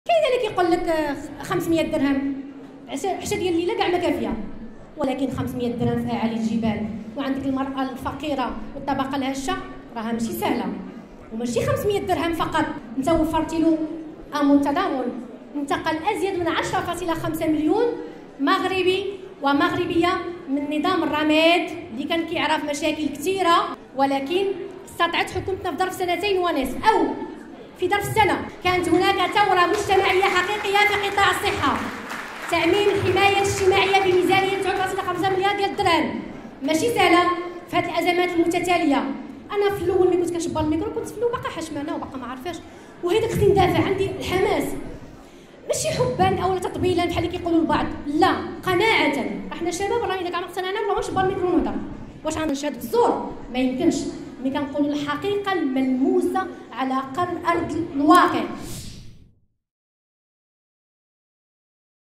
السيمو: حصيلة الحكومة غنية وغير قابلة للتلخيص لكثرة المنجزات
نوهت زينب السيمو، رئيسة المنظمة الجهوية للمرأة التجمعية بجهة طنجة تطوان الحسيمة، خلال المنتدى الإقليمي السادس للشبيبة التجمعية بإقليم العرائش، بنجاح الحكومة في تنزيل البرامج والأوراش الاجتماعية، خصوصا الدعم المباشر والتغطية الصحية، على الرغم من الأزمات المتتالية.